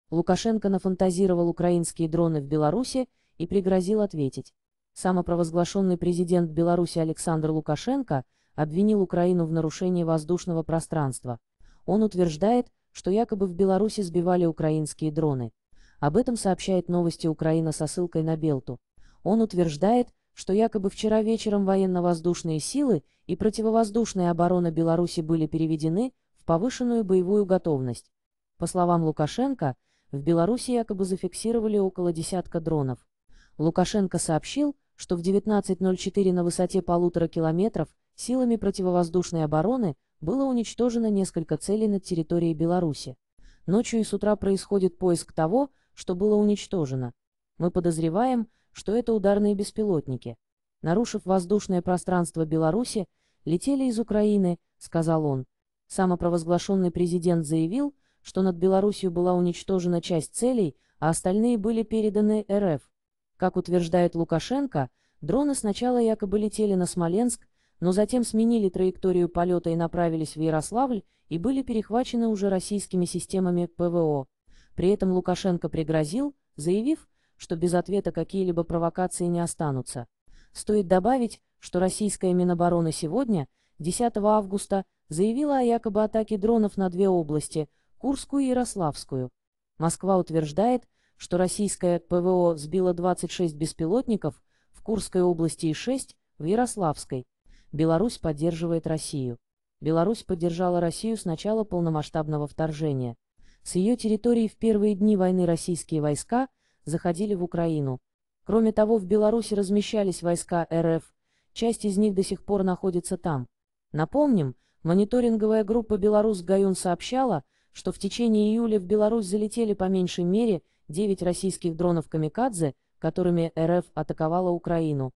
Лукашенко нафантазировал "украинские дроны" в Беларуси и пригрозил ответить.
АлександрЛукашенко РоссийскаяФедерация Беларусь Дрони Самопровозглашенный президент Беларуси Александр Лукашенко обвинил Украину в нарушении воздушного пространства. Он утверждает, что якобы в Беларуси сбивали украинские дроны. Об этом сообщает Новости Украина со ссылкой на "Белту". Он утверждает, что якобы вчера вечером военно-воздушные силы и противовоздушная оборона Беларуси были переведены в повышенную боевую готовность. По словам Лукашенко, в Беларуси якобы зафиксировали около десятка дронов. Лукашенко сообщил, что в 19:04 на высоте полутора кил